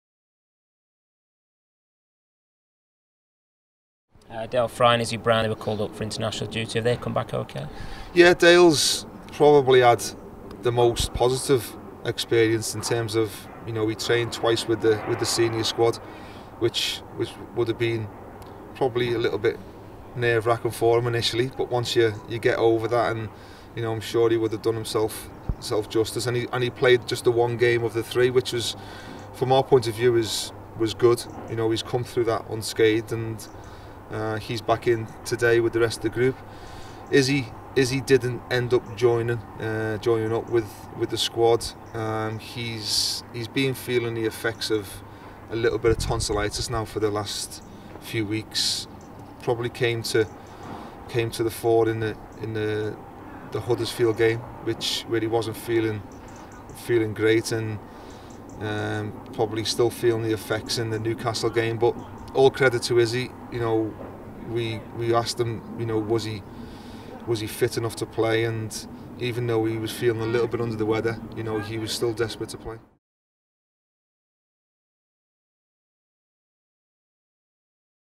YOUTUBE | Alan Stubbs on Dael Fry's experience with the England senior squad
Rotherham United manager Alan Stubbs on Middlesbrough loanee Dael Fry's experience with the England senior squad.